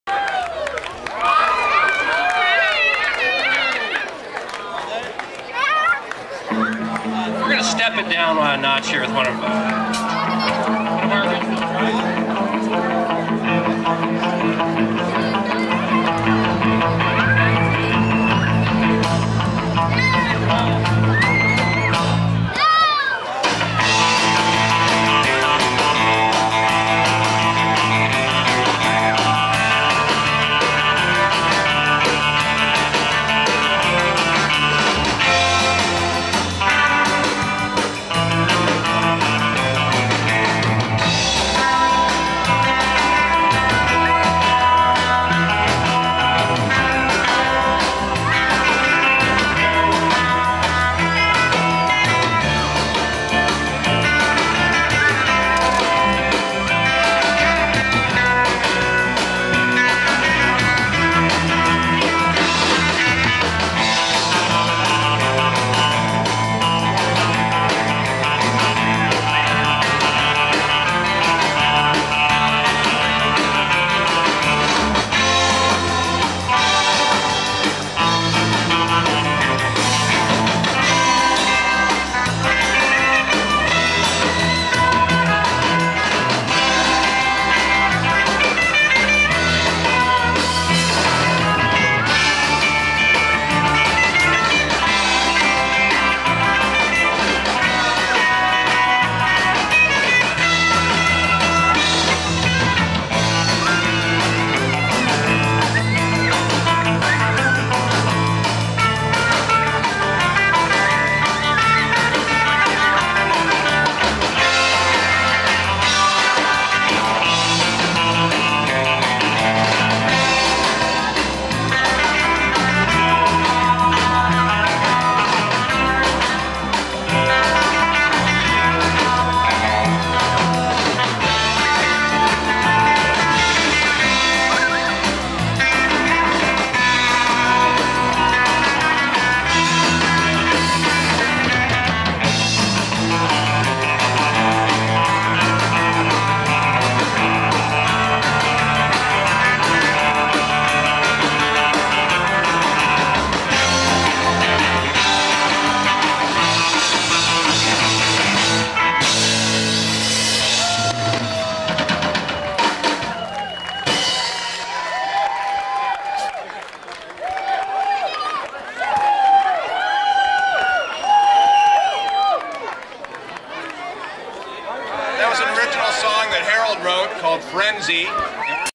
15Frenzy.dv
Surf band North Swell live at Fog Fest, Pacifica, CA. Sept. 24, 2000.